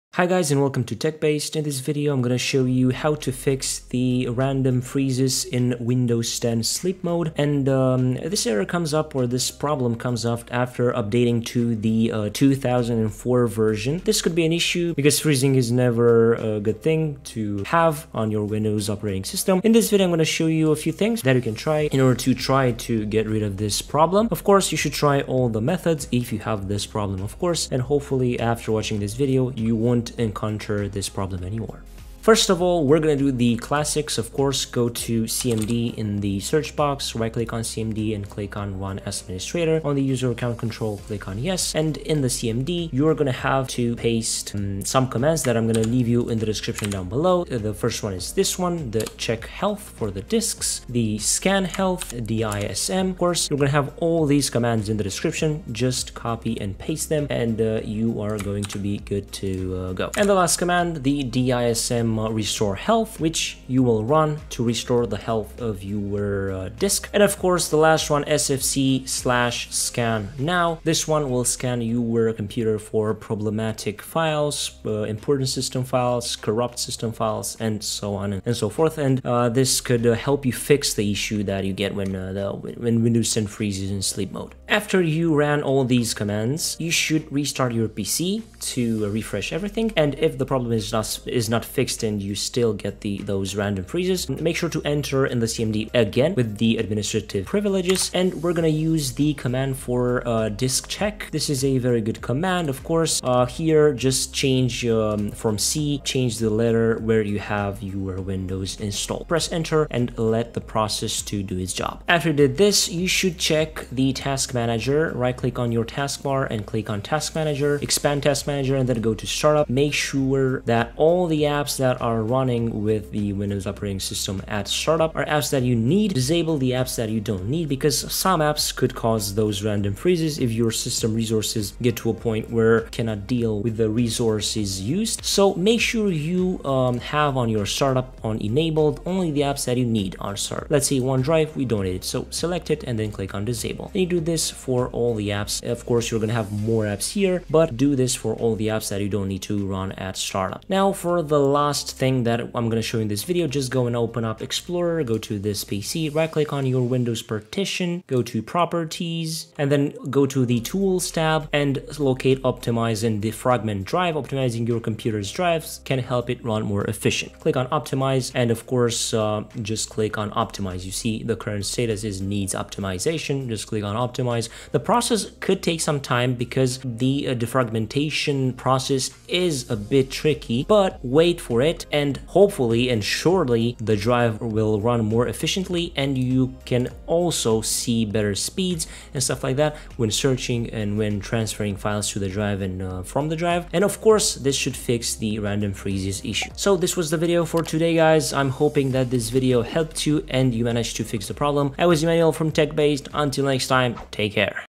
Fix: Windows 10 2004 Random Freezing in Sleep Mode
In this video I'm going to show you how to Fix: Windows 10 2004 Random Freezing in Sleep Mode

Commands used in the video :
sfc /scannow
chkdsk C: /f